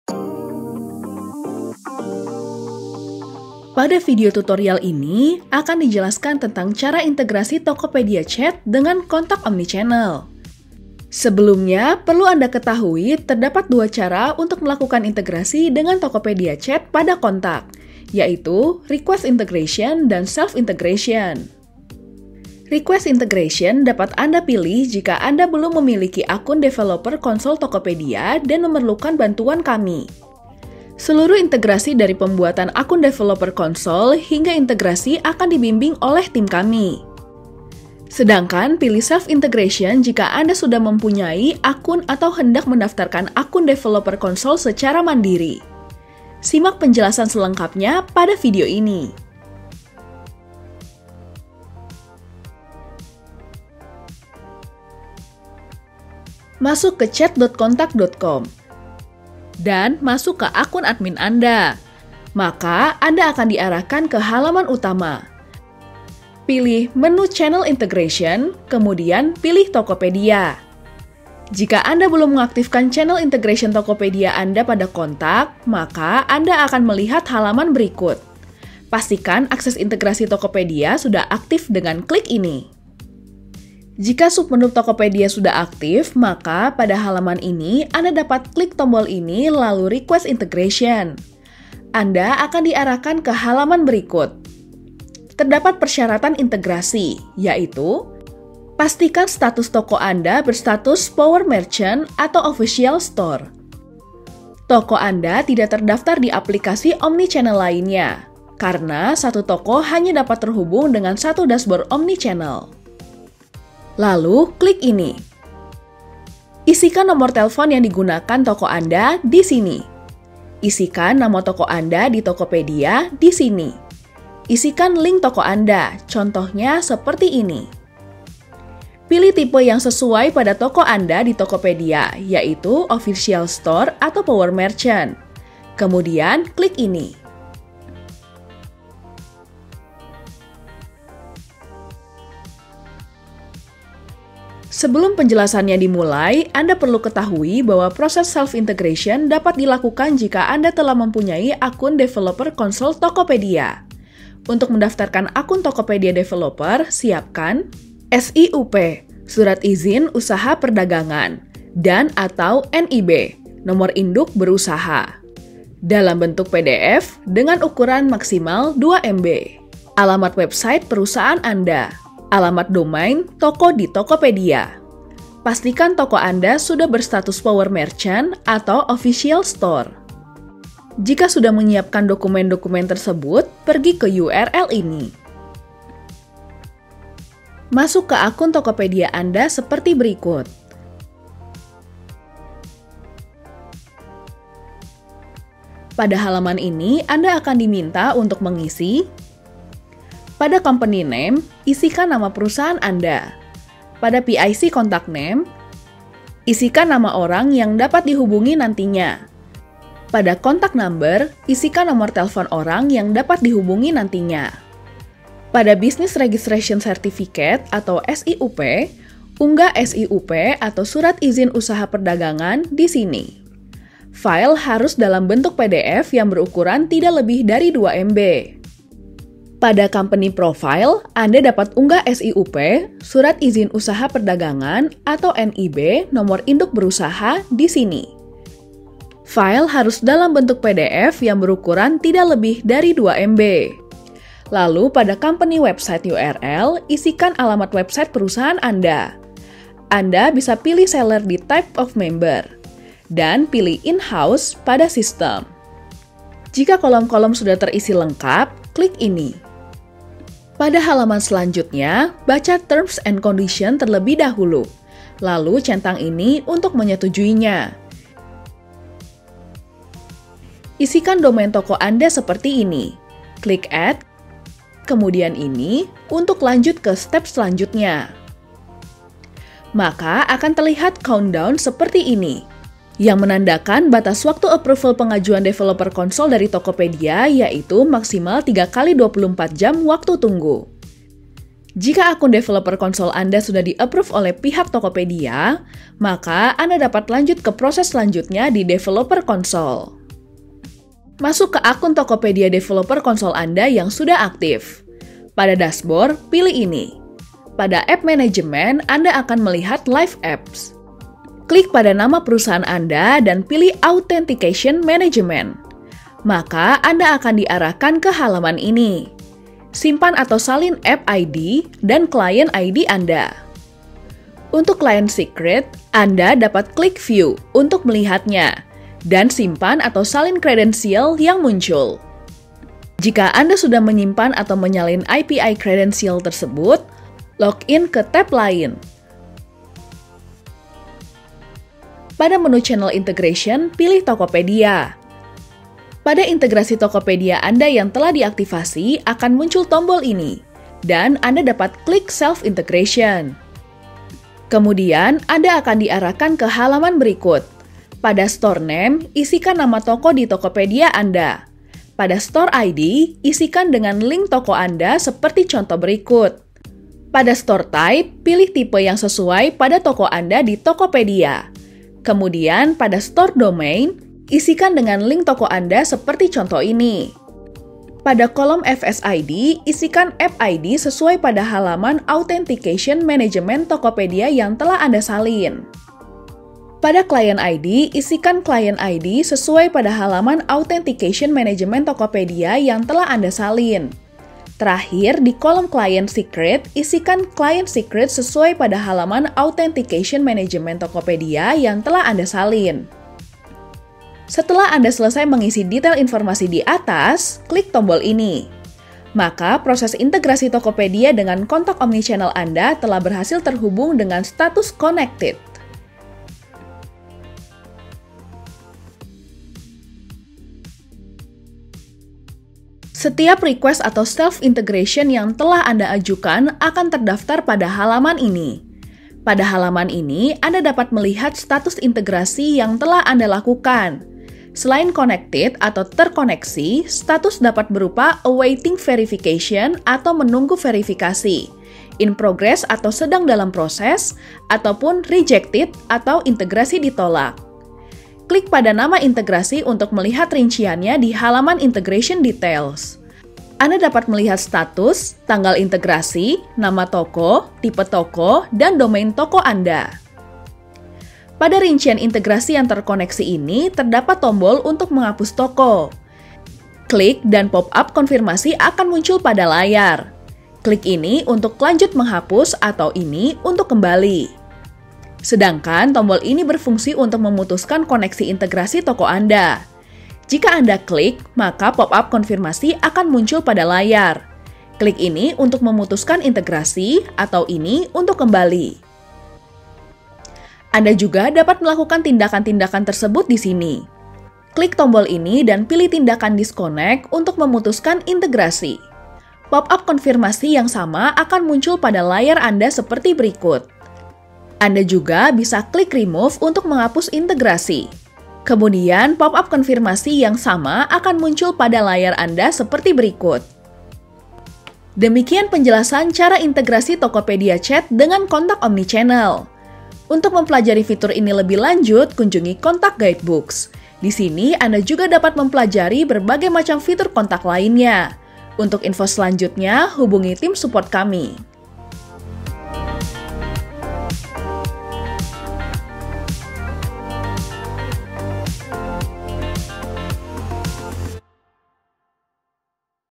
Tutorial - Integration: Cara Mengintegrasikan Tokopedia Chat
Qontak merupakan software Omnichannel CRM berbasis web dan Mitra Resmi WhatsApp & Instagram API no. 1 di Indonesia. Omnichannel Qontak dapat menyatukan seluruh channel komunikasi populer dunia ke dalam 1 platform. Salah satu channelnya adalah Tokopedia. Anda dapat mengintegrasikan Tokopedia chat dengan Qontak Omnichannel untuk kemudahan transaksi bisnis Anda. Pelajari selengkapnya melalui video berikut.

Chapter:
0:52 Cara Request Integration Tokopedia Chat
2:20 Cara Self Integration Tokopedia Chat
6:57 Halaman Daftar Integrasi Tokopedia
Accelerate Sales and Customer Service with Mekari Qontak

Baca tutorial seputar Aplikasi CRM Qontak: help-center.qontak.com